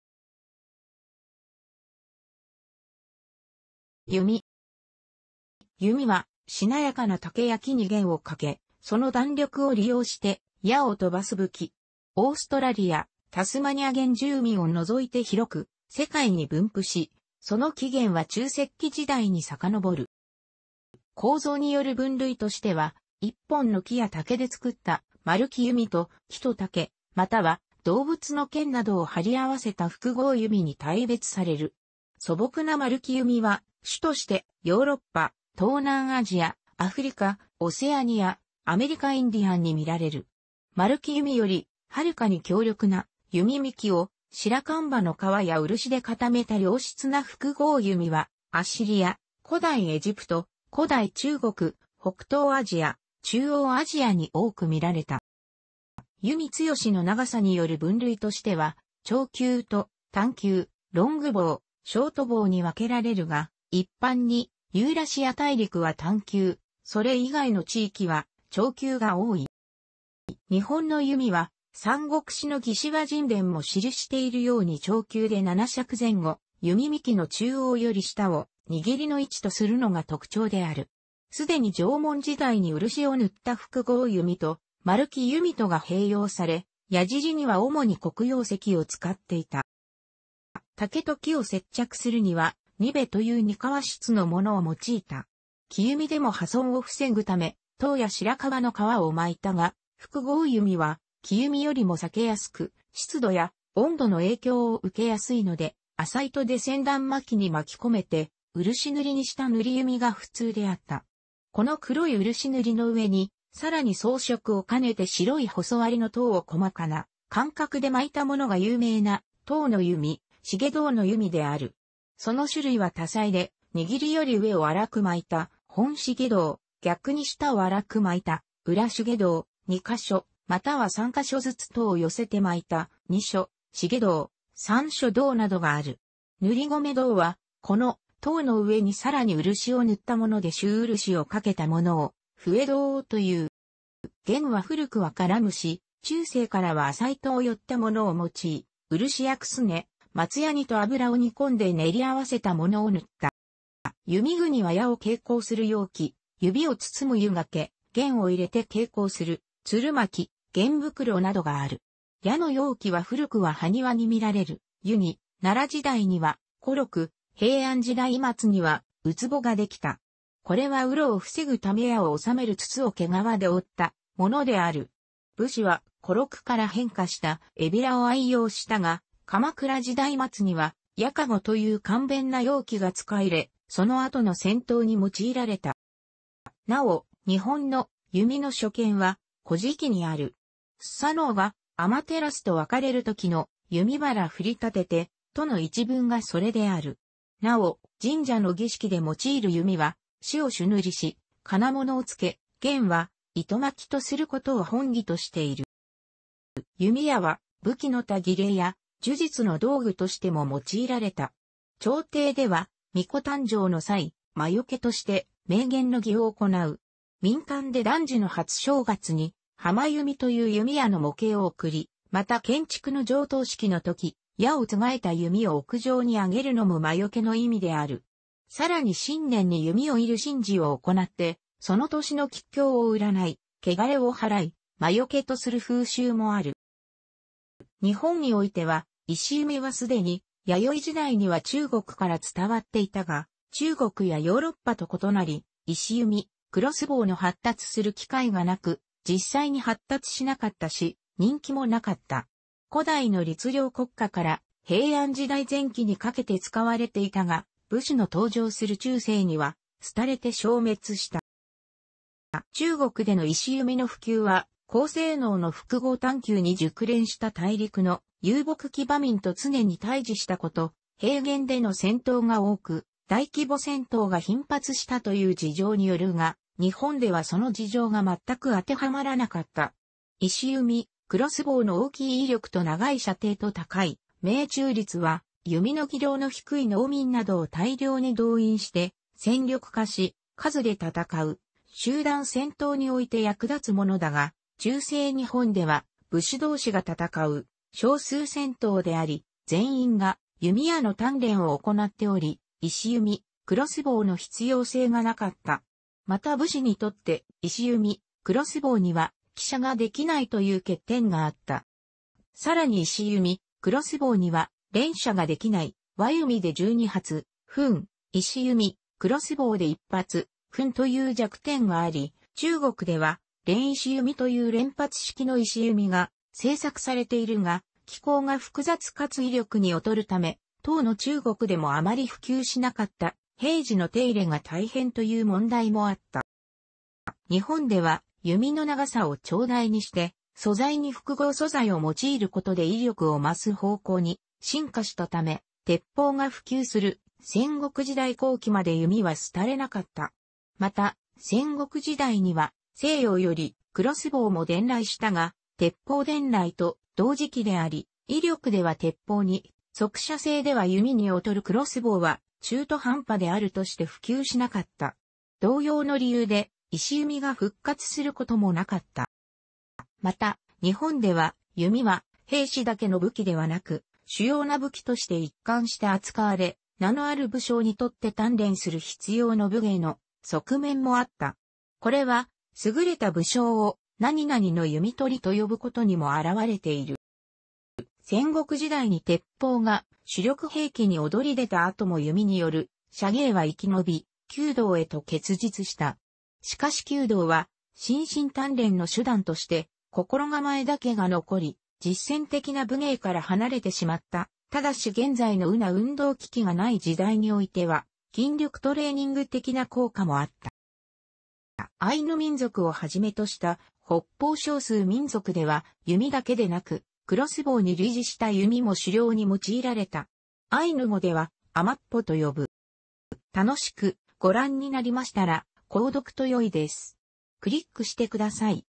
弓 (武器)
弓（ゆみ）はしなやかな竹や木に弦をかけ、その弾力を利用して矢を飛ばす武器。オーストラリア、タスマニア原住民を除いて広く世界に分布し、その起源は中石器時代に遡る。

構造による分類としては、1本の木や竹で作った丸木弓と、木と竹または動物の腱などを張り合わせた複合弓に大別される。素朴な丸木弓は主としてヨーロッパ、東南アジア、アフリカ、オセアニア、アメリカインディアンに見られる。丸木弓よりはるかに強力な、弓幹をシラカンバの皮や漆で固めた良質な複合弓は、アッシリア、古代エジプト、古代中国、北東アジア、中央アジアに多く見られた。

弓幹の長さによる分類としては、長弓と短弓（ロングボウ、ショートボウ）に分けられるが、一般にユーラシア大陸は短弓、それ以外の地域は長弓が多い。

日本の弓は三国志の魏志倭人伝も記しているように長弓で7尺前後、弓幹の中央より下を握りの位置とするのが特徴である。既に縄文時代に漆を塗った複合弓と丸木弓とが併用され、鏃には主に黒曜石を使っていた。

竹と木を接着するには「にべ」というニカワ質のものを用いた。木弓でも破損を防ぐ為トウやシラカバの皮を巻いたが、複合弓は木弓よりも裂けやすく、湿度や温度の影響を受けやすいので、麻糸で千段巻に巻き込めて漆塗りにした塗弓が普通であった。この黒い漆塗りの上にさらに装飾をかねて白い細割のトウを細かな間隔で巻いた物が有名な「重籐の弓」（しげどうのゆみ）である。その種類は多彩で、握りより上を荒く巻いた「本重籐」、逆に下を荒く巻いた「裏重籐」、2箇所、または3箇所ずつトウを寄せて巻いた「二所重籐」「三所重籐」などがある。「塗籠籐」はこのトウの上にさらに漆を塗ったもので朱漆をかけたものを「笛籐」という。

弦は古くはカラムシ、中世からは麻糸をよったものを用い、漆やクスネ（松脂と油を煮込んで練り合わせたもの）を塗った。

弓具には矢を携行する容器、指を包むユガケ、弦を入れて携行する弦巻（弦袋）などがある。矢の容器は古くは埴輪にみられる「靫」、奈良時代には「コロク」、平安時代末には「空穂」ができた。これは雨露を防ぐ為矢を収める筒を毛皮で覆ったものである。武士はコロクから変化した「箙」を愛用したが、鎌倉時代末には「矢籠」という簡便な容器が使われ、その後の戦闘に用いられた。

なお、日本の「弓」の初見は古事記にある。スサノオがアマテラスと別れる時の「弓腹振り立てて…」との一文がそれである。なお、神社の儀式で用いる弓は、梓を朱塗りし、金物を附け、弦は糸巻とする事を本義としている。

弓矢は武器の他儀礼や呪術の道具としても用いられた。朝廷では皇子誕生の際、魔よけとして鳴弦の儀を行う。民間で男児の初正月に破魔弓という弓矢の模型を贈り、また建築の上棟式のとき、矢を番えた弓を屋上に上げるのも魔よけの意味である。さらに新年に弓を射る神事を行って、その年の吉凶を占い、穢れを祓い魔除けとする風習もある。

日本においては、弩は既に弥生時代には中国から伝わっていたが、中国やヨーロッパと異なり、弩・クロスボウの発達する機会が無く、実際に発達しなかったし、人気も無かった。古代の律令国家から平安時代前期にかけて使われていたが、武士の登場する中世には廃れて消滅した。

中国での弩の普及は、高性能の複合短弓に熟練した大陸の遊牧騎馬民と常に対峙した事、平原での戦闘が多く大規模戦闘が頻発したという事情によるが、日本ではその事情が全くあてはまらなかった。弩・クロスボウの大きい威力と長い射程と高い命中率は、弓の技量の低い農民などを大量に動員して戦力化し、数で戦う集団戦闘において役立つものだが、中世日本では武士同士が戦う小数戦闘であり、全員が弓矢の鍛錬を行っており、弩・クロスボウの必要性が無かった。また武士にとって、弩・クロスボウには騎射ができないという欠点があった。

さらに弩・クロスボウには連射ができない（和弓で12発/分、弩・クロスボウで1発/分）という弱点があり（中国では連弩という連発式の弩が製作されているが、機構が複雑かつ威力に劣るため、当の中国でもあまり普及しなかった）、平時の手入れが大変という問題もあった。

日本では弓の長さを長大にして、素材に複合素材を用いる事で威力を増す方向に進化したため、鉄砲が普及する戦国時代後期まで弓は廃れなかった。また戦国時代には西洋よりクロスボウも伝来したが、鉄砲伝来と同時期であり、威力では鉄砲に、速射性では弓に劣るクロスボウは、中途半端であるとして普及しなかった。同様の理由で弩が復活することもなかった...